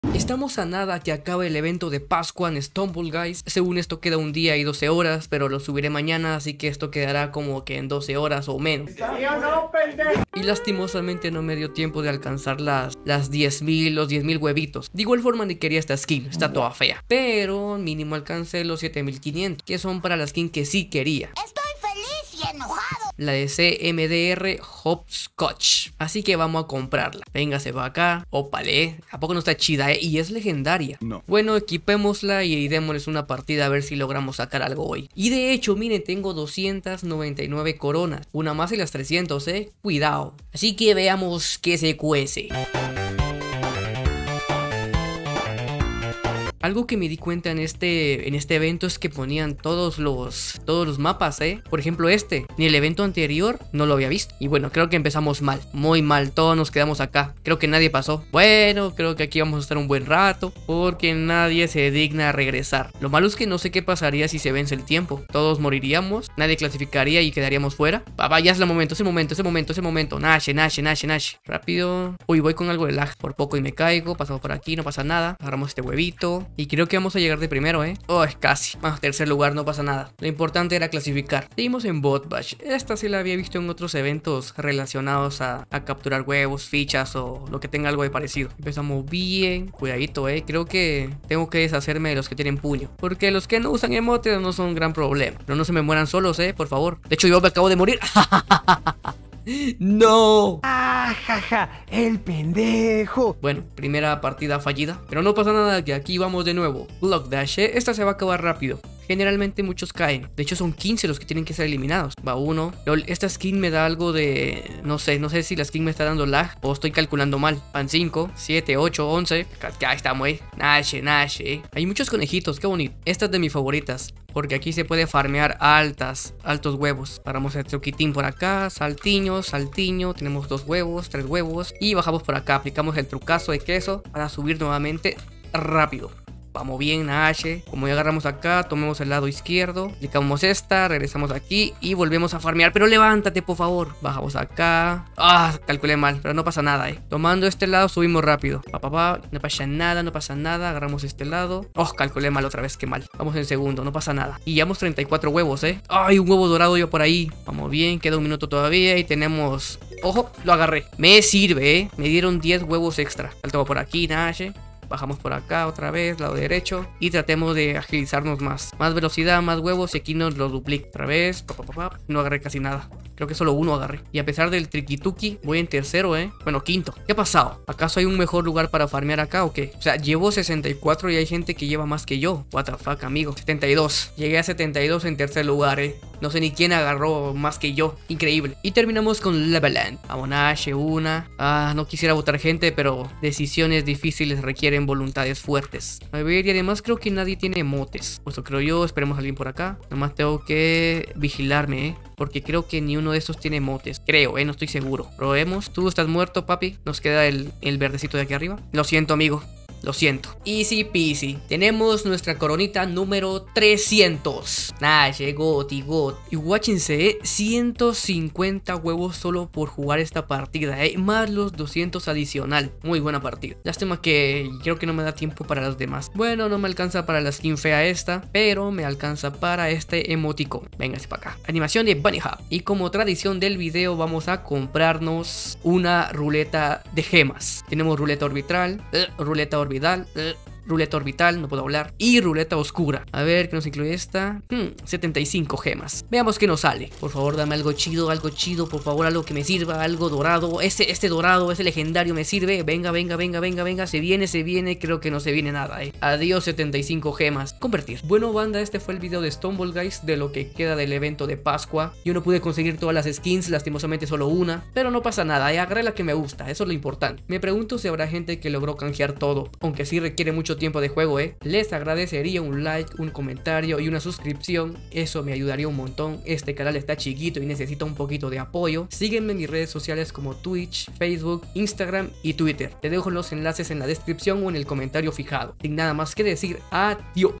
🥚 Alto evento de pascua y +300 Coronas 👑🤑 | DIAMG
No pude conseguir todas las skins pero al menos conseguí la que más me gustaba, ¿alguien si consiguió todas?  😁